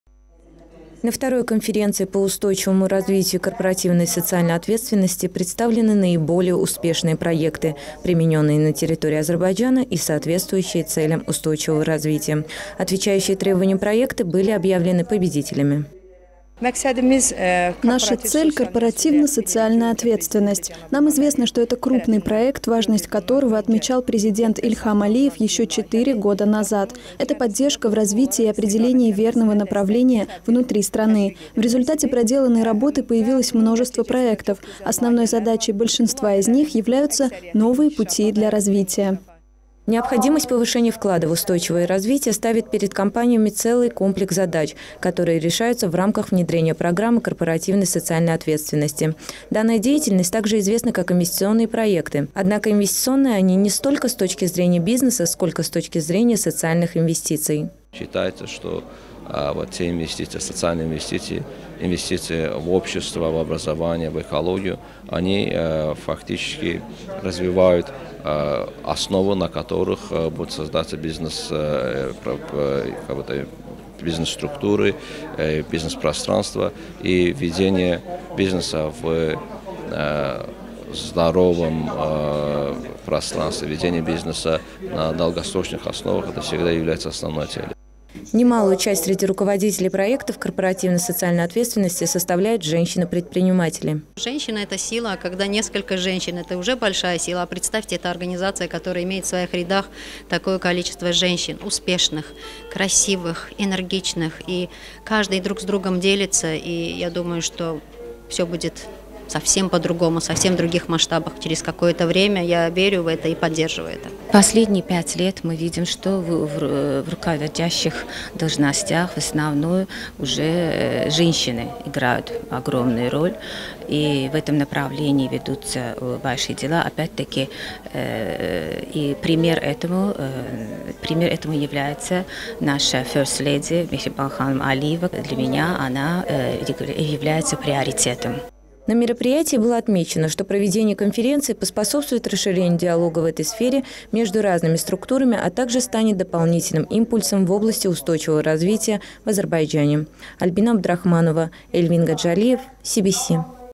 В Баку прошла конференция по устойчивому развитию и корпоративной социальной ответственности
Корпоративная социальная ответственность в современном мире является важной частью ведения бизнеса. Немалую часть руководителей проектов, соответствующих целям устойчивого развития, составляют женщины-предпринимательницы.

На второй конференции по устойчивому развитию и корпоративной социальной ответственности были представлены наиболее успешные проекты, реализуемые на территории Азербайджана и соответствующие целям устойчивого развития. Отвечающие этим требованиям проекты были объявлены победителями.